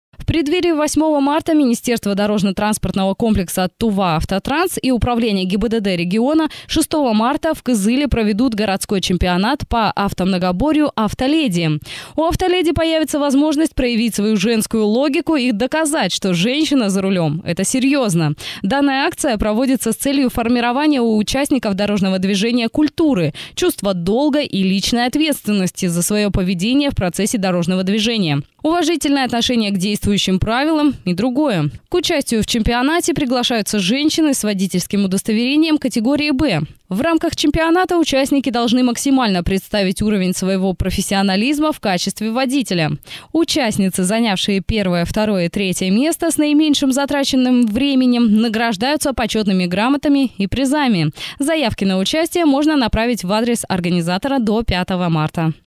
«Автоледи 2019»
В преддверии 8 Марта Министерство дорожно-транспортного комплекса, «Туваавтотранс» и Управление ГИБДД региона 6 марта в Кызыле проведут Городской Чемпионат по автомногоборью «Авто – леди».